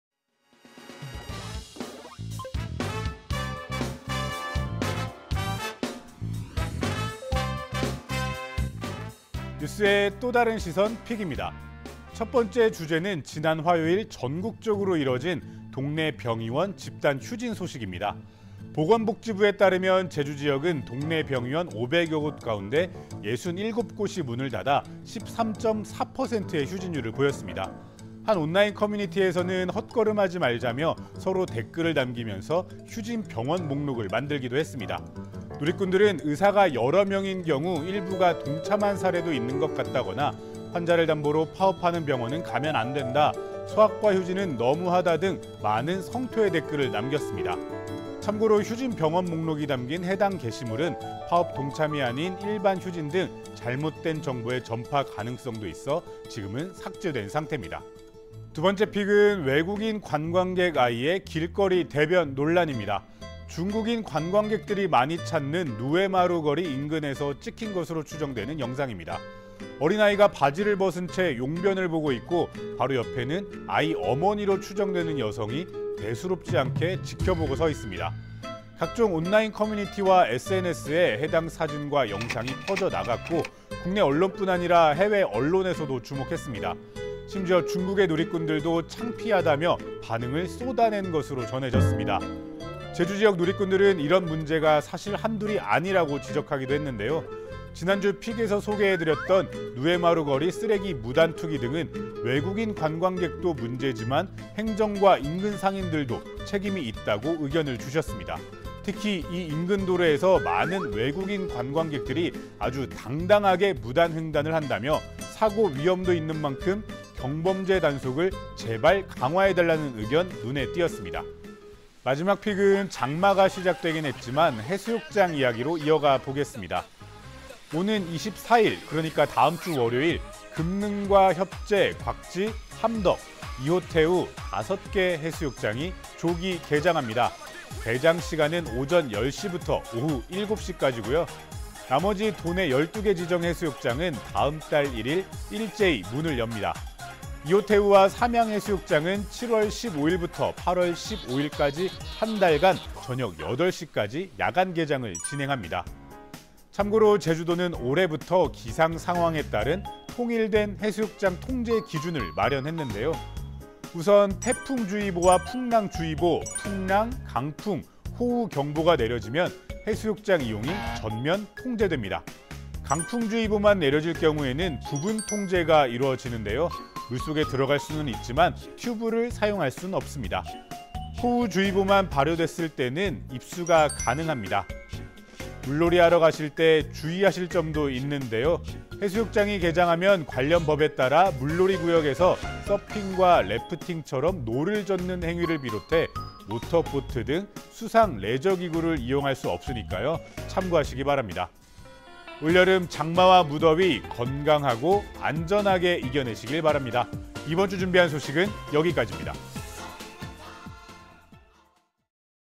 PICK
뉴스의 또다른 시선 pick입니다.

- 제주 병원 휴진 리스트

첫번째 주제는 지난 화요일 전국적으로 이뤄진 동네 병의원 집단 휴진 소식 입니다.

보건복지부에 따르면 제주 지역은 동네 병의원 500여 곳 가운데 67곳이 문을 닫아 13.4%의 휴진율을 보였습니다.

한 온라인 커뮤니티에서는 헛걸음 하지 말자며 서로 댓글을 남김면서 휴진 병원 목록을 만들기도 했습니다.

누리꾼들은 의사가 여러 명인 경우 일부가 동참한 사례도 있는 것 같다거나 환자를 담보로 파업하는 병원은 가면 안된다, 소아과 휴진은 너무하다 등 많은 성토의 댓글을 남겼습니다.

참고로 휴진 병원 목록이 담긴 해당 게시물은 파업 동참이 아닌 일반 휴진 등 잘못된 정보의 전파 가능성도 있어 지금은 삭제된 상태입니다.

- 외국인 관광객 경범죄 스멀스멀

두번째 픽은 외국인 관광객 아이의 길거리 대변 논란입니다.

중국인 관광객들이 많이 찾는 누웨마루 거리 인근에서 찍힌 것으로 추정되는 영상입니다.

어린 아이가 바지를 벗은 채 용변을 보고 있고 바로 옆에는 아이 어머니로 추정되는 여성이 대수롭지 않게 지켜보고 서 있습니다.

각종 온라인 커뮤니티와 SNS에 해당 사진과 영상이 퍼져나갔고 국내언론뿐 아니라 해외 언론에서도 주목했습니다.

심지어 중국의 누리꾼들도 창피하다며 반응을 쏟아낸 것으로 전해졌습니다.

제주지역 누리꾼들은 이런 문제가 사실 한둘이 아니라고 지적하기도 했는데요.

지난주 픽에서 소개해드렸던 누웨마루 거리 쓰레기 무단투기 등은 외국인 관광객도 문제지만 행정과 인근 상인들도 책임이 있다고 의견을 주셨습니다.

특히 이 인근 도로에서 많은 외국인 관광객들이 아주 당당하게 무단횡단을 한다며 사고 위험도 있는만큼 경범죄 단속을 제발 강화해달라는 의견 눈에 띄었습니다.

- 다음주 해수욕장 개장

마지막 픽은 장마가 시작되긴 했지만 해수욕장 이야기로 이어가 보겠습니다.

오는 24일, 그러니까 다음주 월요일 금능과 협재, 곽지, 함덕, 이호테우 5개 해수욕장이 조기 개장합니다.

개장 시간은 오전 10시부터 오후 7시까지고요.

나머지 도내 지정 해수욕장은 다음달 1일 일제히 문을 엽니다.

이호테우와 삼양해수욕장은 7월 15일부터 8월 15일까지 한 달간 저녁 8시까지 야간개장을 진행합니다.

참고로 제주도는 올해부터 기상 상황에 따른 통일된 해수욕장 통제기준을 마련했는데요.

우선 태풍주의보와 풍랑주의보, 풍랑.강풍.호우 경보가 내려지면 해수욕장 이용이 전면 통제됩니다.

강풍주의보만 내려질 경우에는 부분통제가 이뤄지는데요.

물 속에 들어갈 수는 있지만 튜브를 사용해서는 안됩니다.

호우주의보가 발효될 때는 정상 운영됩니다.

물놀이 하러 가실 때 주의하실 점도 있는데요.

해수욕장이 개장하면 관련법에 따라 물놀이구역에서 서핑과 래프팅처럼 노를 젓는 행위를 비롯해 모터보트 등 수상레저기구를 이용할 수 없으니 참고하시기 바랍니다.

올여름 장마와 무더위 건강하고 안전하게 이겨내시길 바랍니다.

이번주 준비한 소식은 여기까지입니다.